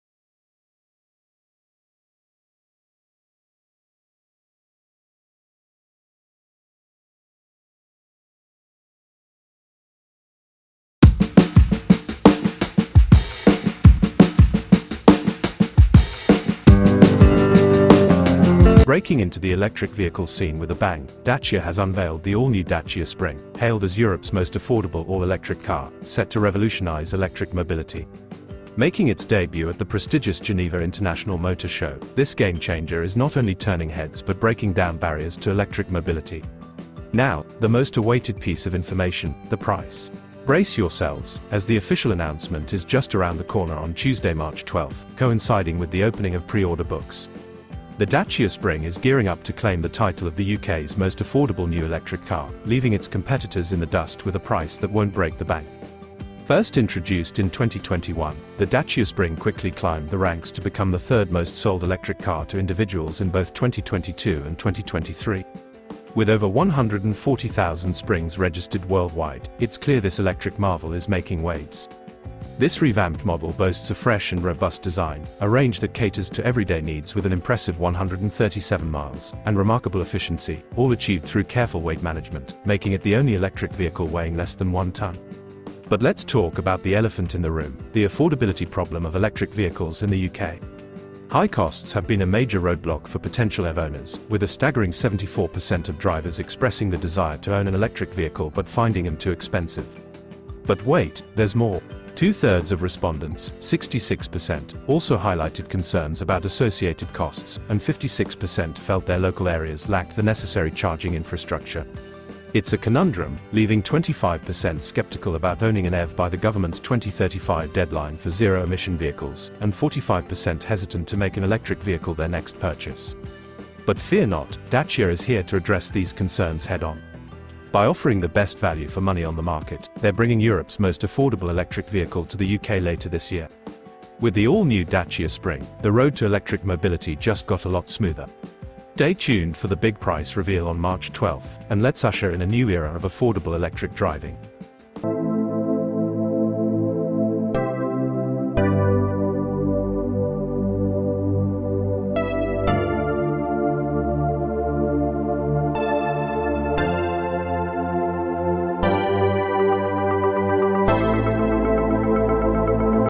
Dacia Spring: Revolutionizing Electric Mobility with Europe's Most Affordable EV!
Discover the future of electric driving with the All-New Dacia Spring! Unveiled at the Geneva International Motor Show, this game-changing electric vehicle is set to redefine affordability in the UK market. Explore the revamped design, impressive range of 137 miles, and exceptional efficiency – all while remaining the most affordable fully-electric model. Don't miss the chance to be part of the electric revolution with the Dacia Spring.